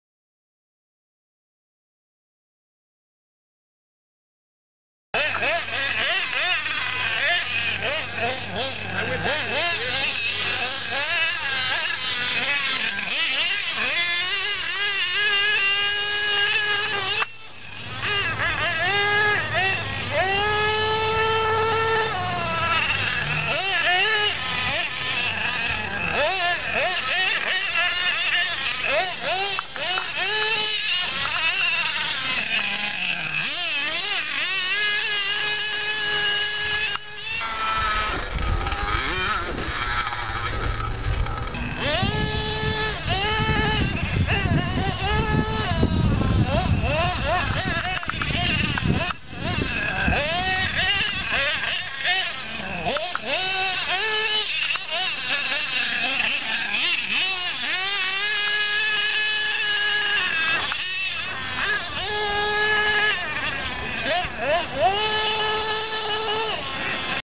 Nitro Rc - Fun at the Sandlot Outdoor Track
A couple laps from today. We had Nitro and Electric RC's out to bash